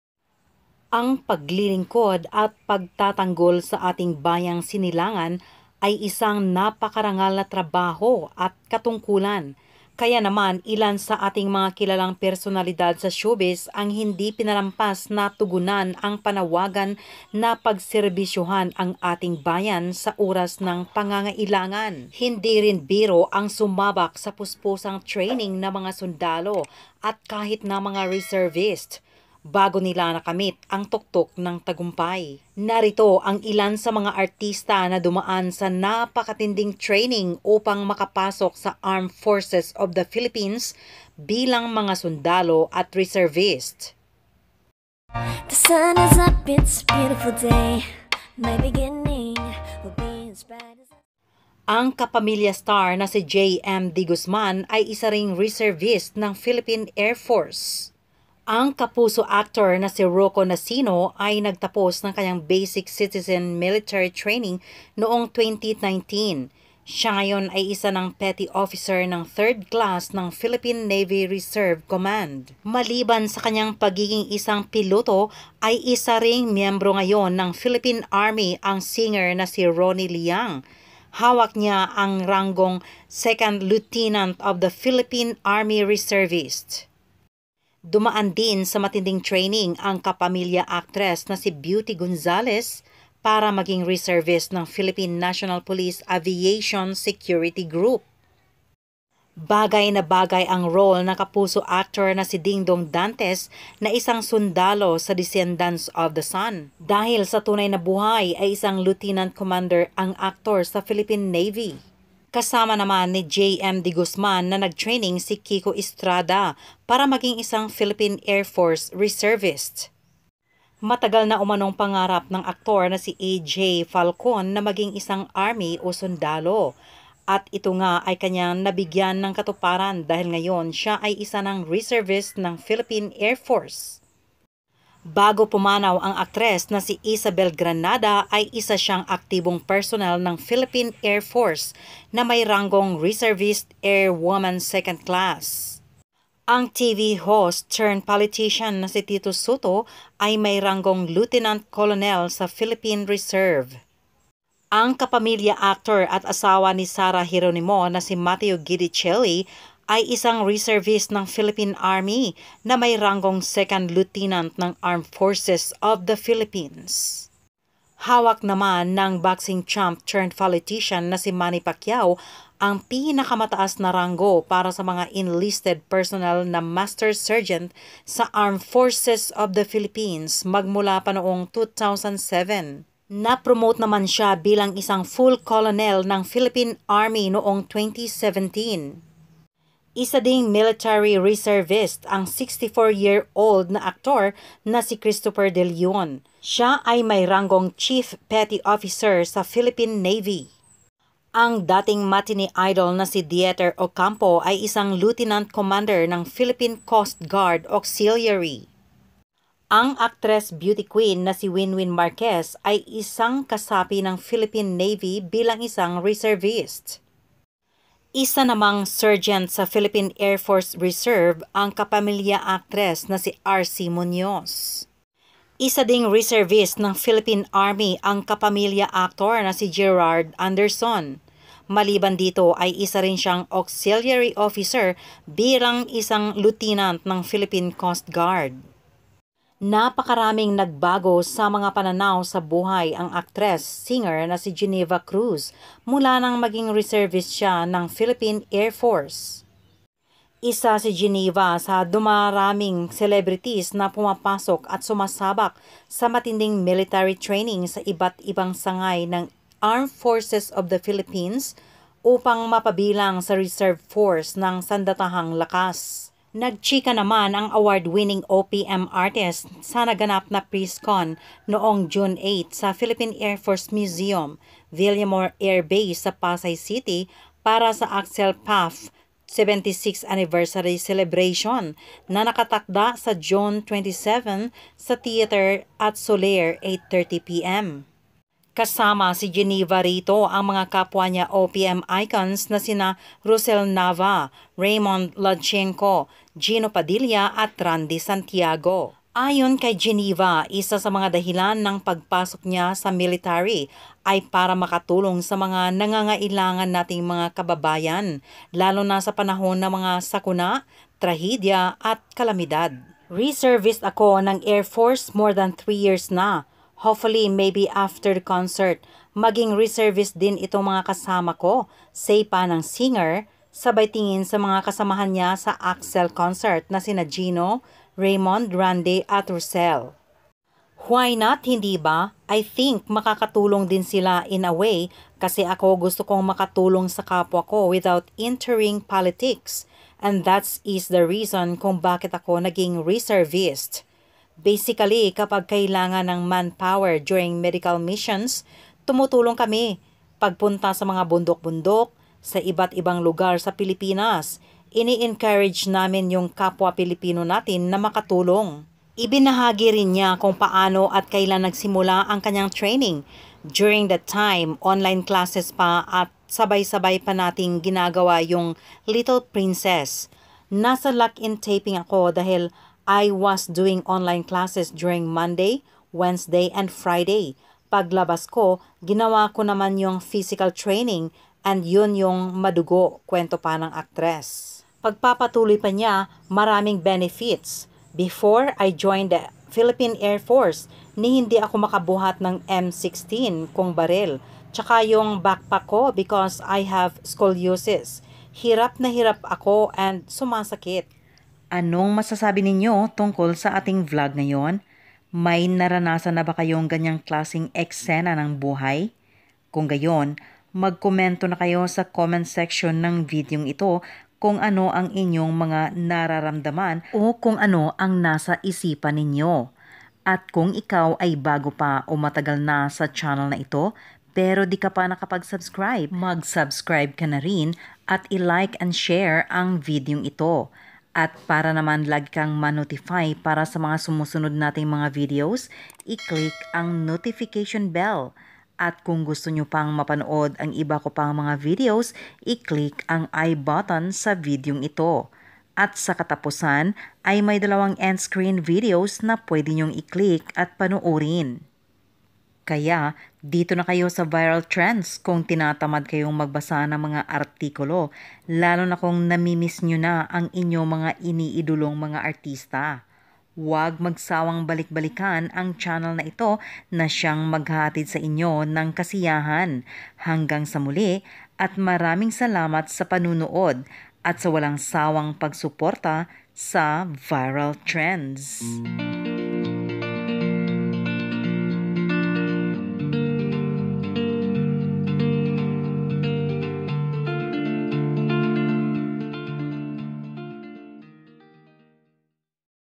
Mga sundalong dati ay artista nakaharap sa camera || Bibilib ka sa tapang nila sa bakbakan
Alamin ang mga artistang matapang na pinasok ang pagiging sundalo. Sumabak sa posposang training, kahit mahirap pero tinapos nila ang training.